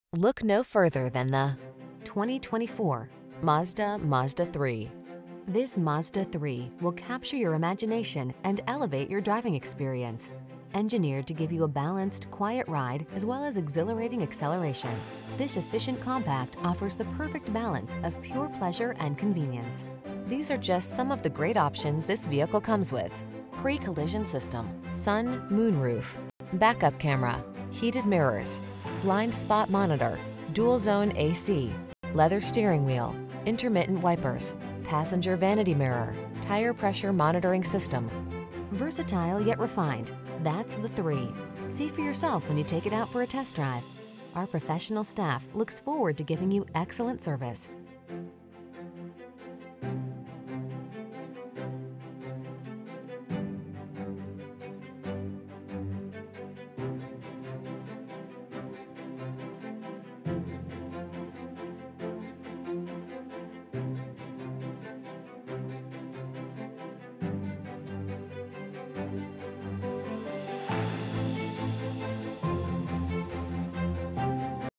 2024 Mazda Mazda3_Sedan 2.5 S Preferred AZ Phoenix, Chandler, Gilbert, Tempe, Maricopa
New 2024 Mazda Mazda3_Sedan 2.5 S Preferred available at Horne Mazda located in 7777 South Test Drive, Tempe, AZ, 85284. Servicing the Phoenix, Chandler, Gilbert, Tempe, Maricopa area.

Look no further than the  2024  MAZDA Mazda3. This Mazda 3 will capture your imagination and elevate your driving experience. Engineered to give you a balanced, quiet ride as well as exhilarating acceleration, this efficient compact offers the perfect balance of pure pleasure and convenience. These are just some of the great options this vehicle comes with: Pre-Collision System, Sun/Moonroof, Back-Up Camera, Heated Mirrors, Blind Spot Monitor, Dual Zone A/C, Leather Steering Wheel, Intermittent Wipers, Passenger Vanity Mirror, Tire Pressure Monitoring System. Versatile yet refined, that's the 3. See for yourself when you take it out for a test drive. Our professional staff looks forward to giving you excellent service!